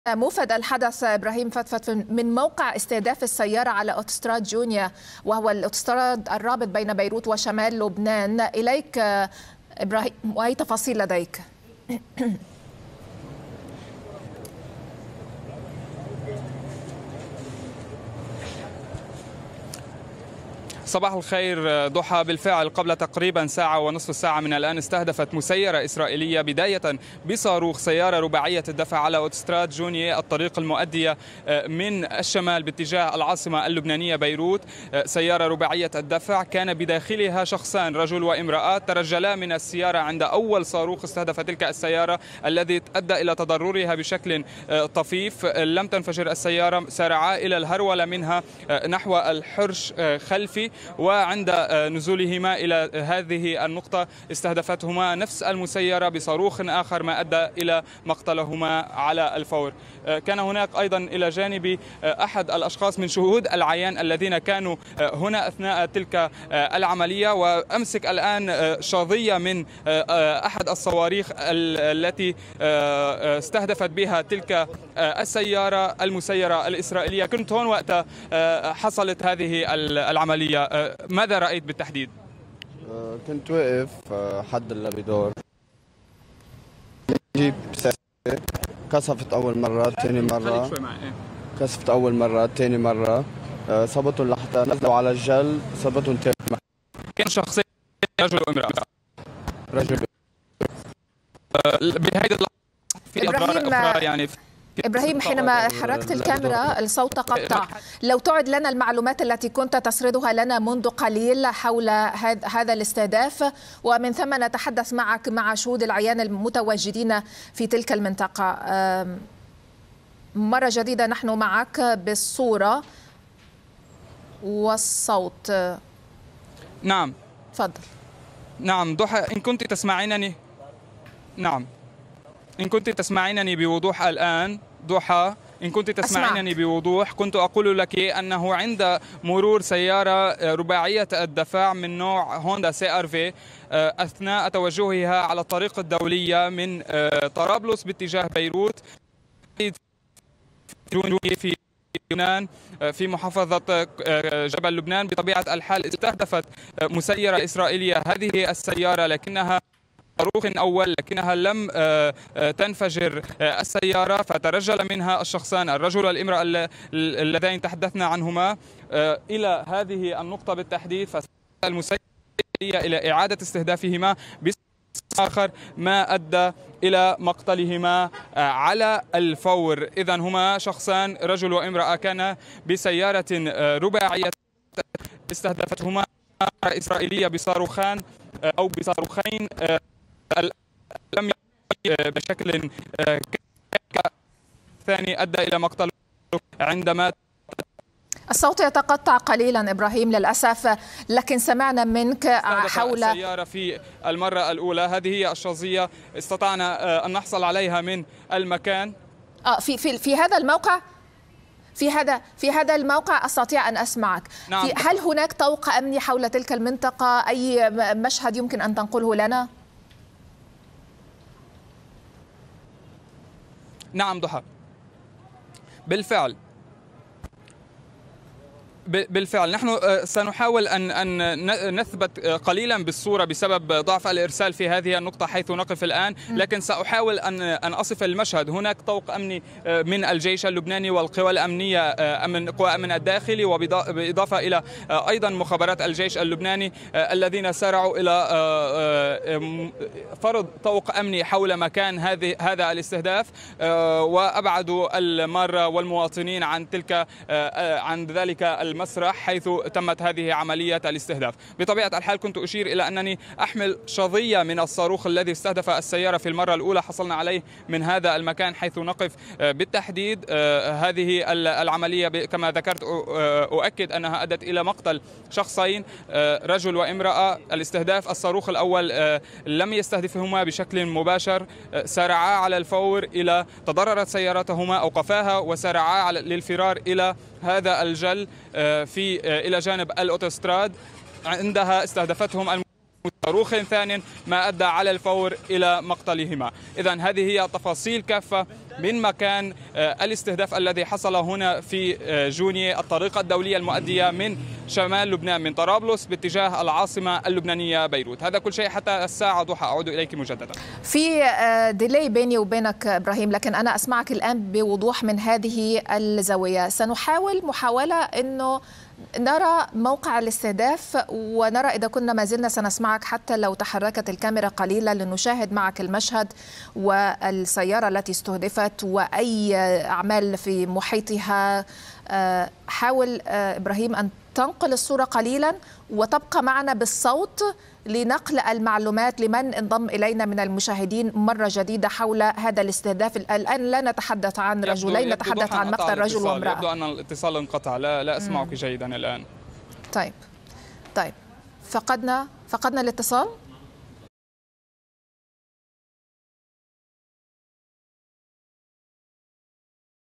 "الحدث" بموقع استهداف السيارة على أوتوستراد جونية الرابط بين بيروت وشمال لبنان
موفد الحدث إبراهيم فتفت: مسيّرة إسرائيلية استهدفت سيارة رباعية الدفع على أوتوستراد جونية بصاروخين كان بداخلها رجل وامرأة تم قتلهما على الفور